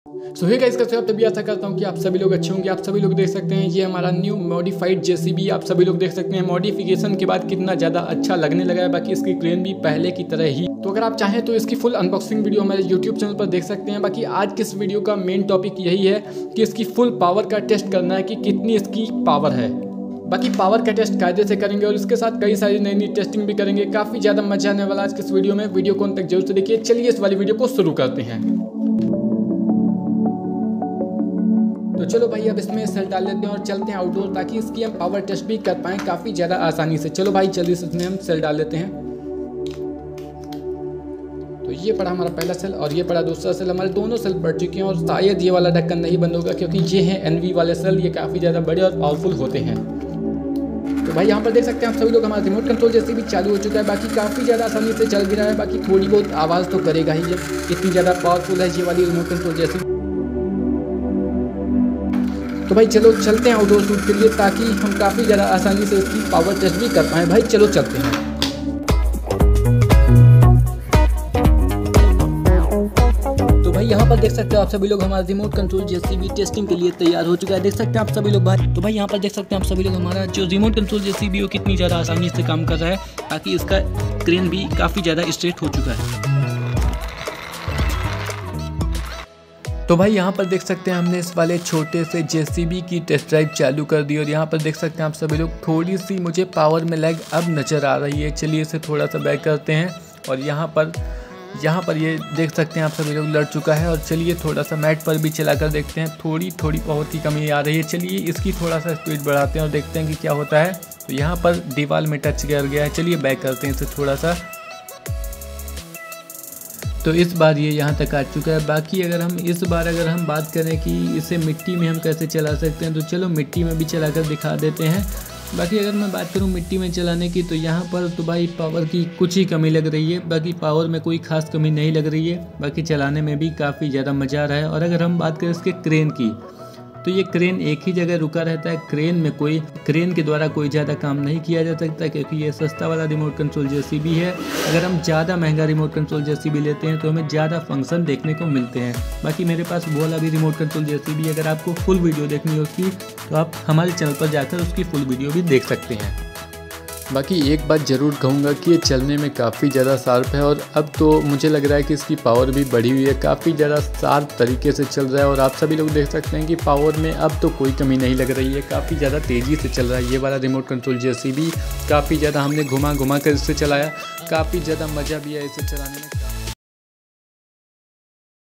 Remote Control Mini Jcb.
In this video we tested a remote control JCB
this very powerful and fast also.